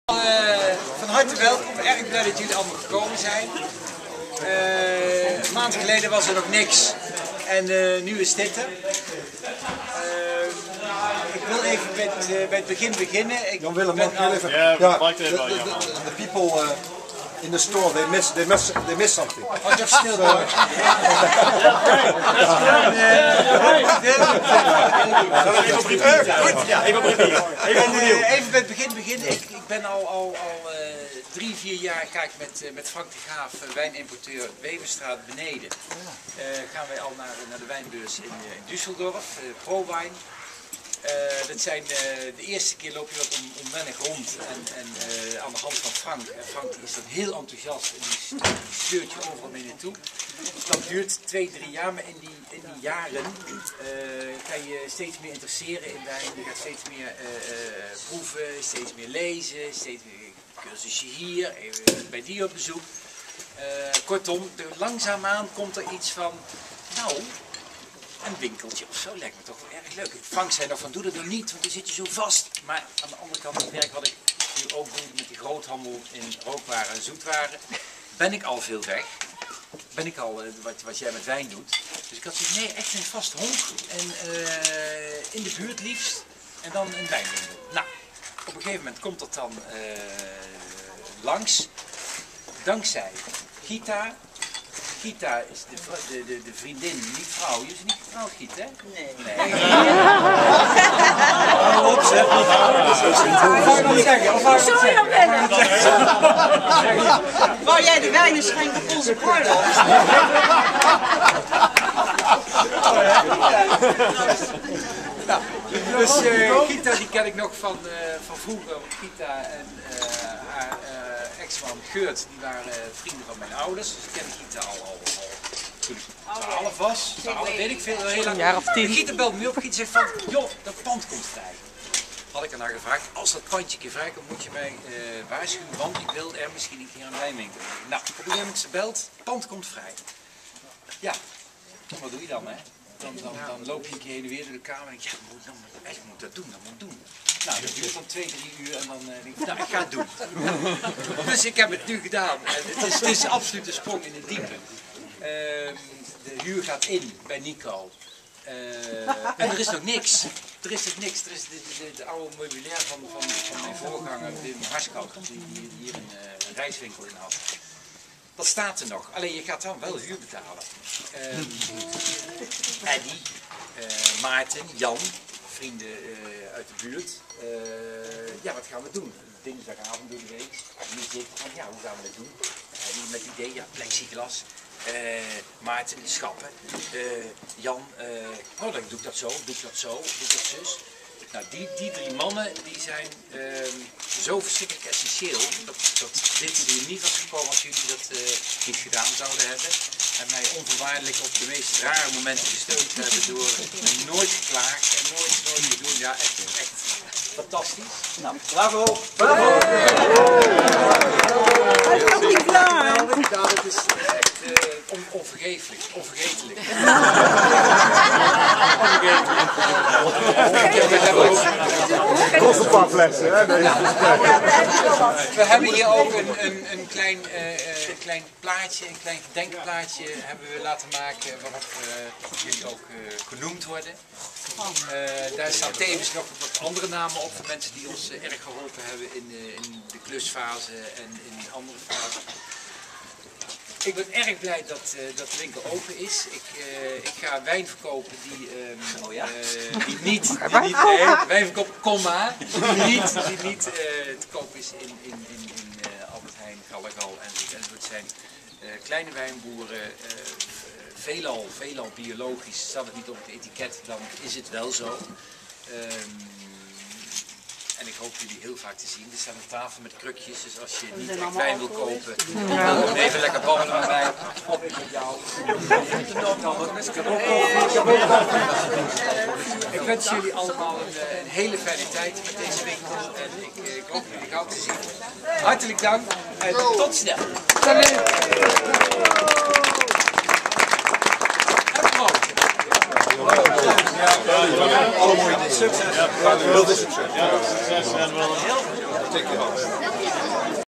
Openingsspeech Van de Wijnen Oosterbeek
Jan Willem vertelt hoe het allemaal zo is gekomen